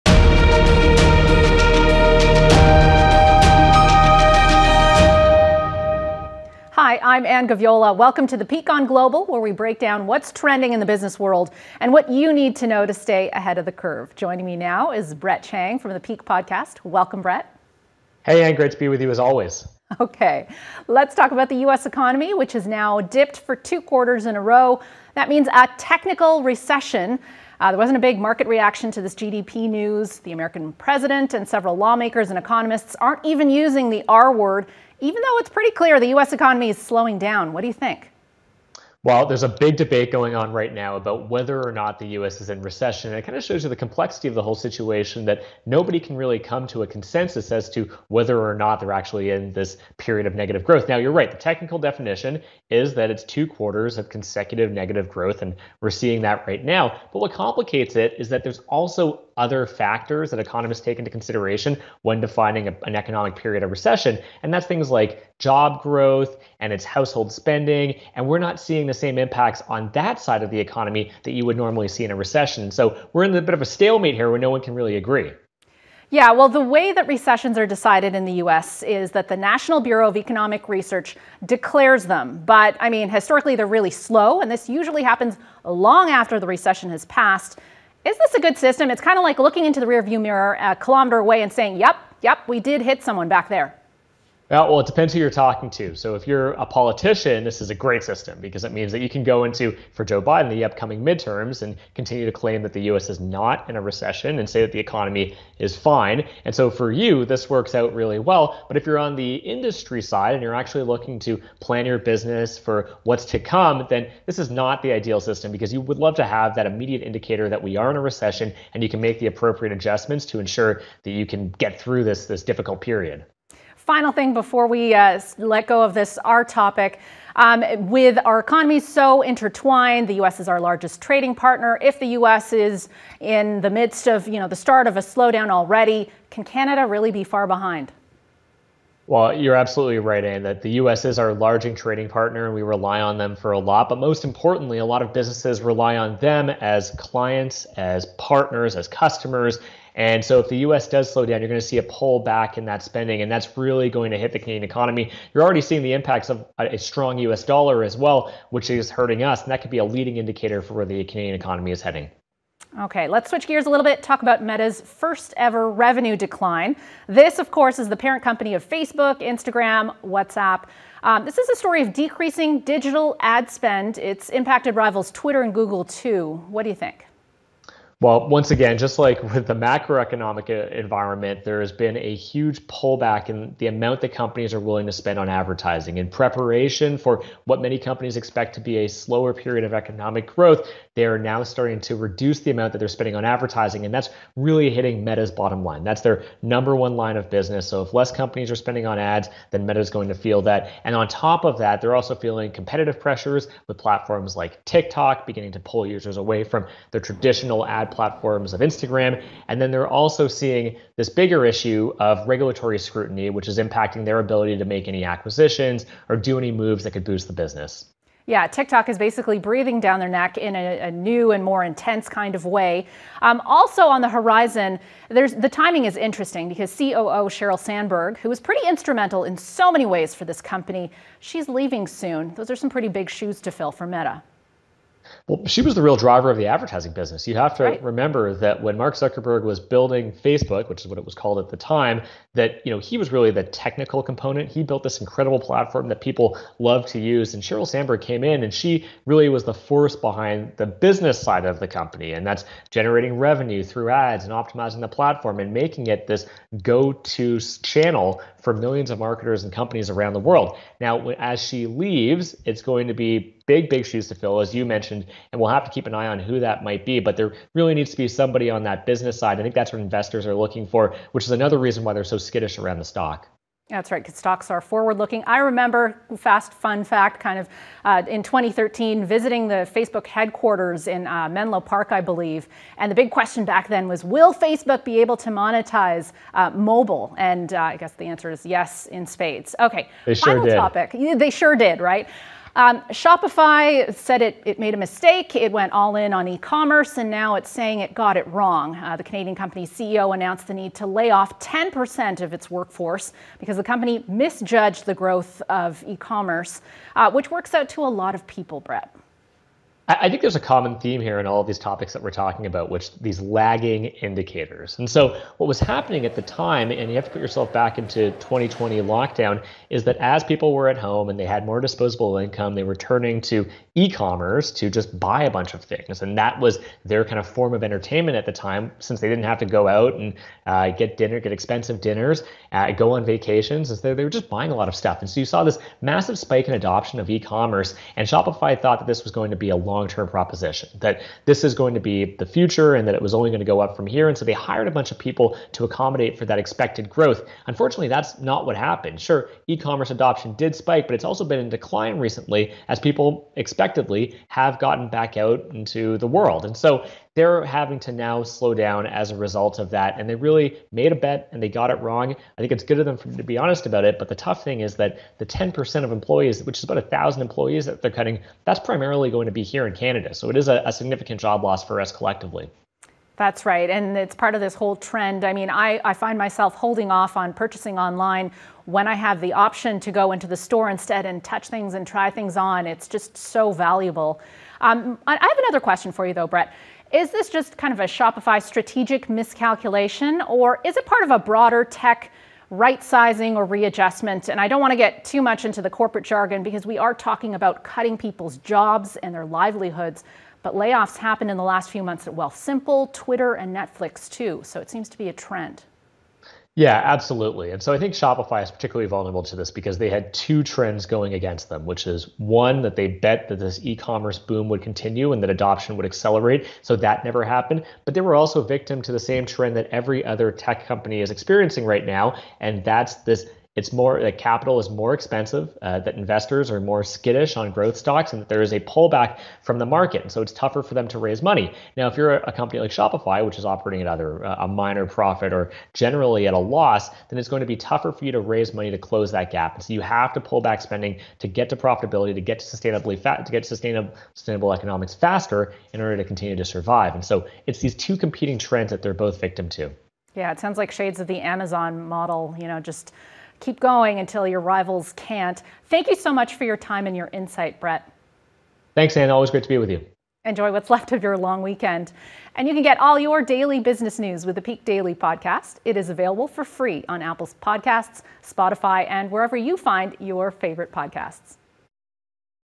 Is the US economy in a recession?
The U.S. economy has dipped for two quarters in a row, meaning the country is in a recession under the traditional definition. However, there hasn’t been a big market reaction to the GDP news, and U.S. President Joe Biden, economists and several lawmakers have been avoiding the term “recession.”

As Brett Chang reports, there’s a big debate right now about whether the U.S. is in a recession, showing the complexity of the situation with no clear consensus.

Though the two-consecutive quarters of downturn in the American economy fit the traditional definition of a recession, what complicates the situation is that there are also other factors when defining an economic period of recession — such as job growth and household spending, which remains steady at the moment.

In the U.S., recessions are officially decided by the National Bureau of Economic Research, though declarations are historically slow. As Anne Gaviola asks, is this the best system - and who does this benefit?

For Canada, if the U.S. is seeing a slowdown in the economy, will its northern neighbour be far behind?

Brett Chang from The Peak Podcast joins Anne Gaviola for more on these topics and much more.